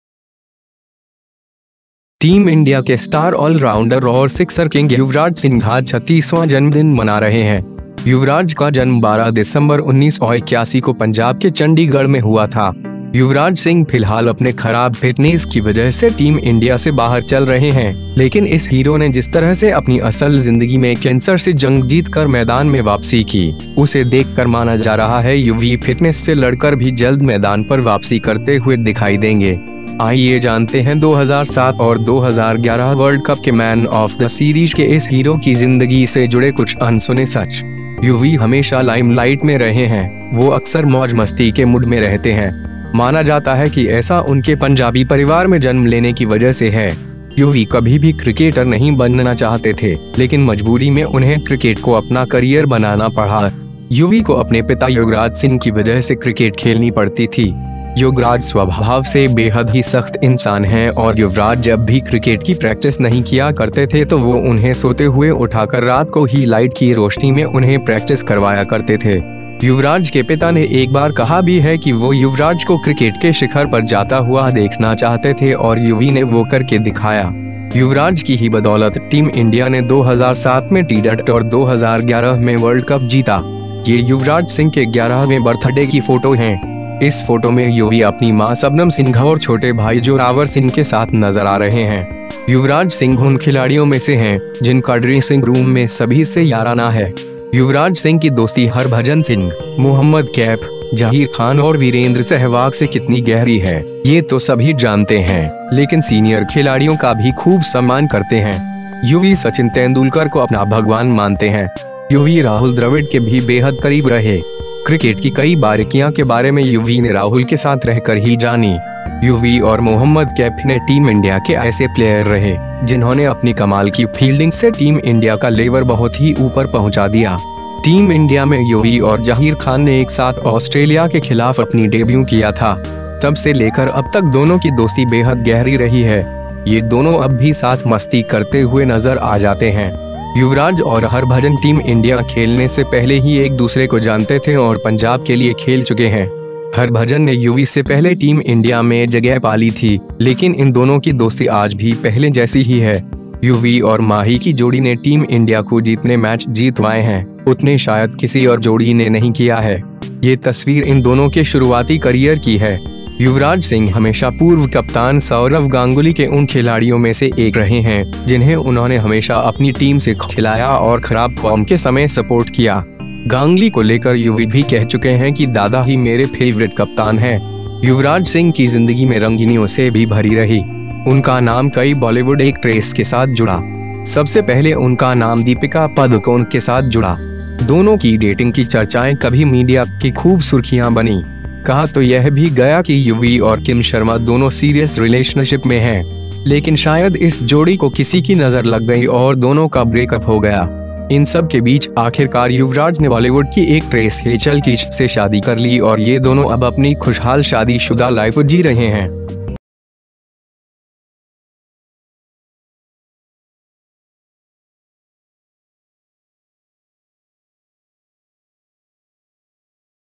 बर्थडे स्पेशलः इन तस्वीरों में छुपा है युवराज का वो हर राज जिसे आप अब तक नहीं जानते
टीम इंडिया के स्टार ऑलराउंडर और सिक्सर किंग युवराज सिंह आज 36वां जन्मदिन मना रहे हैं। युवराज का जन्म 12 दिसंबर 1981 को पंजाब के चंडीगढ़ में हुआ था। युवराज सिंह फिलहाल अपने खराब फिटनेस की वजह से टीम इंडिया से बाहर चल रहे हैं, लेकिन इस हीरो ने जिस तरह से अपनी असल जिंदगी में कैंसर से जंग जीत कर मैदान में वापसी की, उसे देखकर माना जा रहा है युवी फिटनेस से लड़कर भी जल्द मैदान पर वापसी करते हुए दिखाई देंगे। आइए जानते हैं 2007 और 2011 वर्ल्ड कप के मैन ऑफ द सीरीज के इस हीरो की जिंदगी से जुड़े कुछ अनसुने सच...

युवी हमेशा लाइम लाइट में रहे हैं, वो अक्सर मौज-मस्ती के मुड में रहते हैं। माना जाता है कि ऐसा उनके पंजाबी परिवार में जन्म लेने की वजह से है। युवी कभी भी क्रिकेटर नहीं बनना चाहते थे, लेकिन मजबूरी में उन्हें क्रिकेट को अपना करियर बनाना पड़ा।

युवी को अपने पिता योगराज सिंह की वजह से क्रिकेट खेलनी पड़ती थी। योगराज स्वभाव से बेहद ही सख्त इंसान हैं और युवराज जब भी क्रिकेट की प्रैक्टिस नहीं किया करते थे तो वो उन्हें सोते हुए उठाकर रात को ही लाइट की रोशनी में उन्हें प्रैक्टिस करवाया करते थे।

युवराज के पिता ने एक बार कहा भी है कि वो युवराज को क्रिकेट के शिखर पर जाता हुआ देखना चाहते थे और युवी ने वो करके दिखाया। युवराज की ही बदौलत टीम इंडिया ने 2007 में टी20 और 2011 में वर्ल्ड कप जीता।

ये युवराज सिंह के 11वें बर्थडे की फोटो हैं। इस फोटो में युवी अपनी मां सबनम सिंह और छोटे भाई जोरावर सिंह के साथ नजर आ रहे हैं।

युवराज सिंह उन खिलाड़ियों में से हैं, जिनका ड्रेसिंग रूम में सभी से याराना है। युवराज सिंह की दोस्ती हरभजन सिंह, मोहम्मद कैफ, जहीर खान और वीरेंद्र सहवाग से कितनी गहरी है, ये तो सभी जानते हैं, लेकिन सीनियर खिलाड़ियों का भी खूब सम्मान करते हैं। युवी सचिन तेंदुलकर को अपना भगवान मानते हैं। युवी राहुल द्रविड के भी बेहद करीब रहे। क्रिकेट की कई बारिकियां के बारे में युवी ने राहुल के साथ रहकर ही जानी।

युवी और मोहम्मद कैफ ने टीम इंडिया के ऐसे प्लेयर रहे, जिन्होंने अपनी कमाल की फील्डिंग से टीम इंडिया का लेवर बहुत ही ऊपर पहुंचा दिया।

टीम इंडिया में युवी और जहीर खान ने एक साथ ऑस्ट्रेलिया के खिलाफ अपनी डेब्यू किया था। तब से लेकर अब तक दोनों की दोस्ती बेहद गहरी रही है। ये दोनों अब भी साथ मस्ती करते हुए नजर आ जाते हैं।

युवराज और हरभजन टीम इंडिया खेलने से पहले ही एक दूसरे को जानते थे और पंजाब के लिए खेल चुके हैं। हरभजन ने युवी से पहले टीम इंडिया में जगह पा ली थी, लेकिन इन दोनों की दोस्ती आज भी पहले जैसी ही है।

युवी और माही की जोड़ी ने टीम इंडिया को जीतने मैच जीतवाए हैं, उतने शायद किसी और जोड़ी ने नहीं किया है। ये तस्वीर इन दोनों के शुरूआती करियर की है।
युवराज सिंह हमेशा पूर्व कप्तान सौरव गांगुली के उन खिलाड़ियों में से एक रहे हैं, जिन्हें उन्होंने हमेशा अपनी टीम से खिलाया और खराब फॉर्म के समय सपोर्ट किया। गांगली को लेकर युवी भी कह चुके हैं कि दादा ही मेरे फेवरिट कप्तान हैं।

युवराज सिंह की जिंदगी में रंगिनियों से भी भरी रही। उनका नाम कई बॉलीवुड एक्ट्रेस के साथ जुड़ा। सबसे पहले उनका नाम दीपिका पादुकोण के साथ जुड़ा। दोनों की डेटिंग की चर्चाएं कभी मीडिया की खूब सुर्खियां बनीं।

कहा तो यह भी गया कि युवी और किम शर्मा दोनों सीरियस रिलेशनशिप में हैं। लेकिन शायद इस जोड़ी को किसी की नजर लग गई और दोनों का ब्रेकअप हो गया।

इन सब के बीच आखिरकार युवराज ने बॉलीवुड की एक्ट्रेस हेचल कीच से शादी कर ली और ये दोनों अब अपनी खुशहाल शादी-शुदा लाइफ जी रहे हैं।